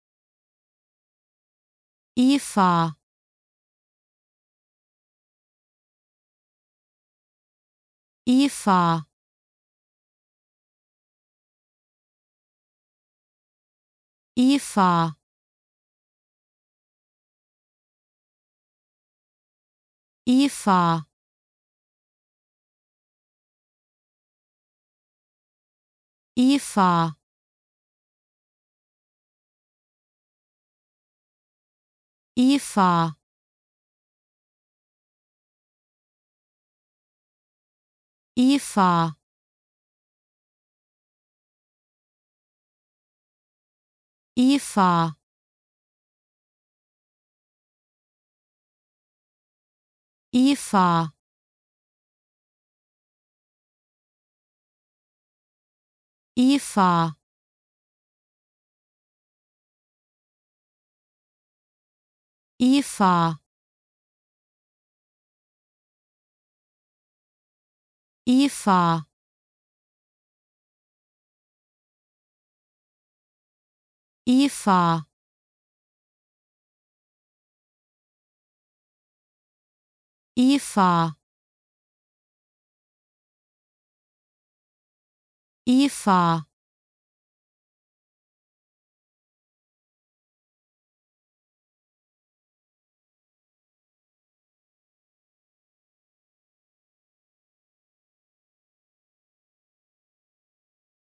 How to Pronounce ''Ifa'' Correctly in German
Learn how to say ''Ifa'' in German with this short pronunciation tutorial.